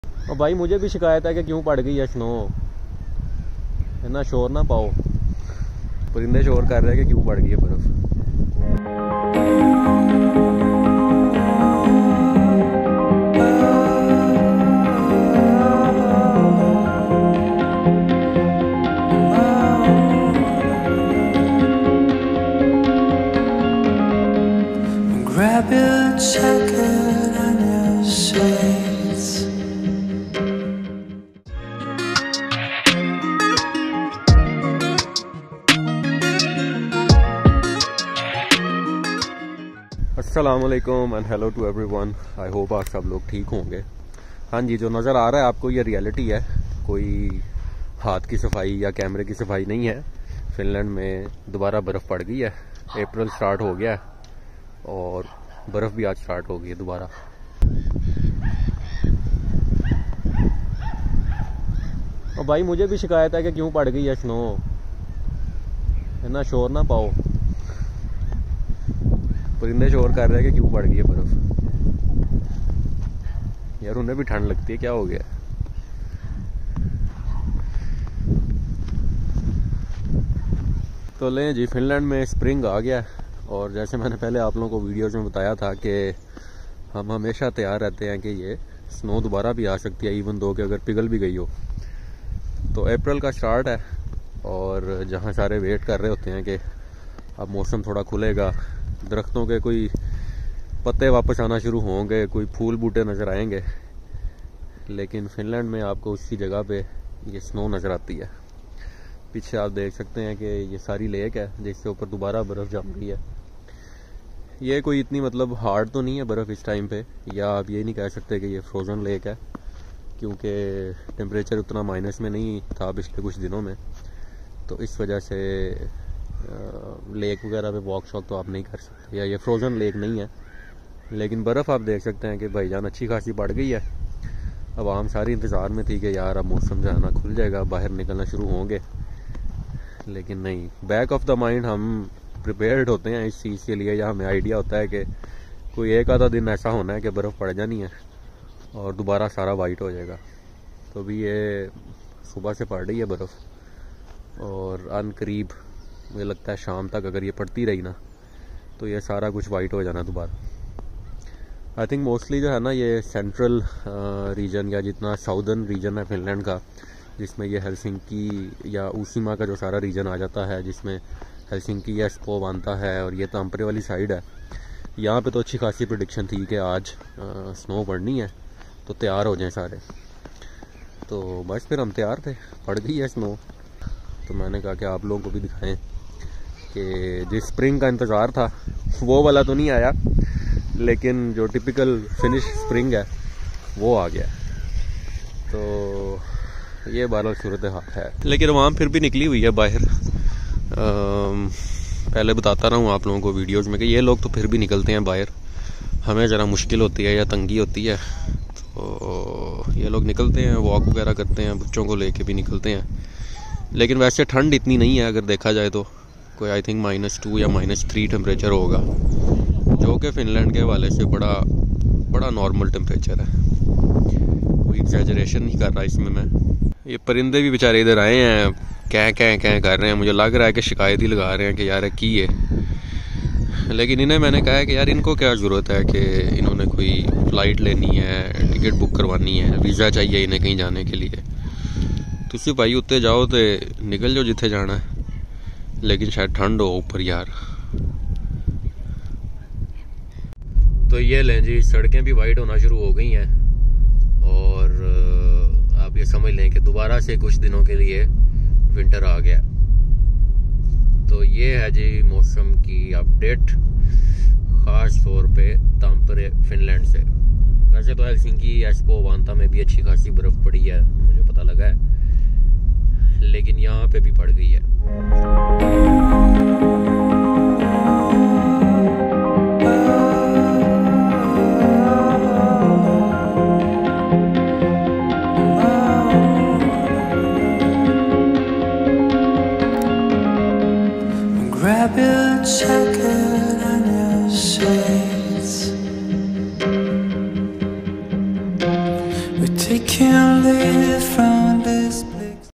Snow is back | Spring in Finland ❄️
Winter is back in Finland with a snowfall lol

People usually ask me about weather conditions in Finland so here you go with an update 😄 We were waiting for spring and it came down to be a typical Finnish spring with a snowfall.